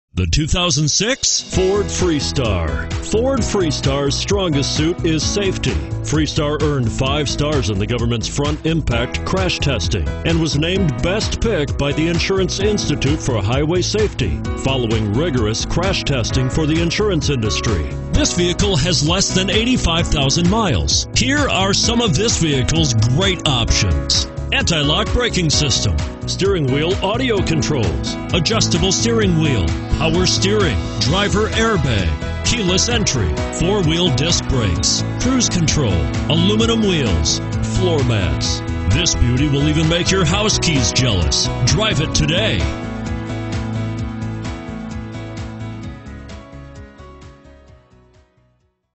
2006 FORD FREESTAR WAGON Cherry Hill, NJ 05783
2006 FORD FREESTAR WAGON Cherry Hill, NJ
Stock# 05783

Don't miss this 2006 Ford Freestar Wagon. It's equipped with Automatic transmission and features a BROWN exterior. With 83575 miles, you'll want to take this car home. Make a great choice today.